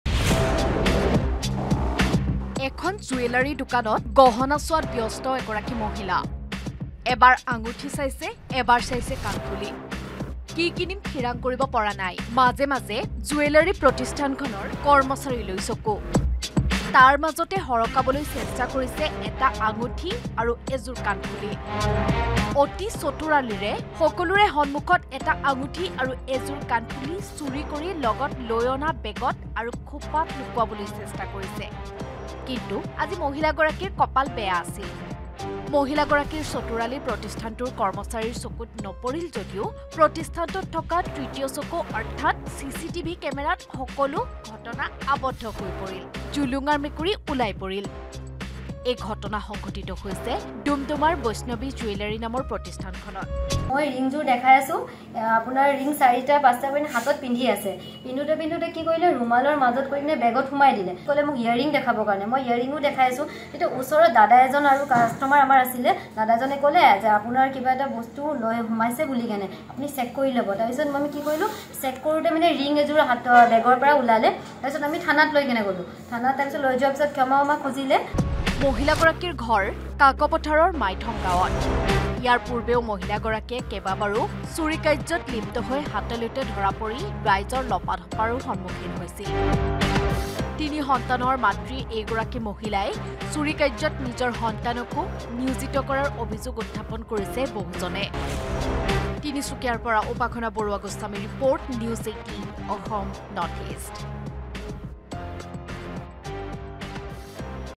Tinsukia Lady Thief : ডুমডুমাৰ জুৱেলাৰী দোকানত হাতেলোতে ধৰিলে মহিলা চোৰক | N18V
Tinsukia Lady Thief | ডুমডুমাৰ জুৱেলাৰী দোকানত মহিলা চোৰক ধৰিলে হাতেলোতে। অলংকাৰ চোৱাৰ চলেৰে সোণৰ আঙুঠি, কাণফুল হৰলুকি কৰাৰ চেষ্টা কৰিছিল। CCTV  কেমেৰাত ধৰা পৰাৰ পাছতে মহিলাগৰাকীক আটক কৰে। ডুমডুমাৰ বৈষ্ণৱী জুৱেলাৰীত এই ঘটনা।